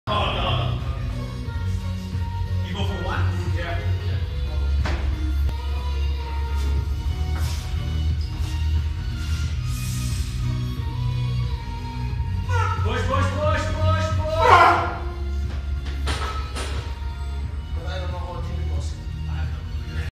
Squats for Strenght - 180kg Kniebeuge + small reverse band
Coach Simon at a 180kg Squat with supporting Bands.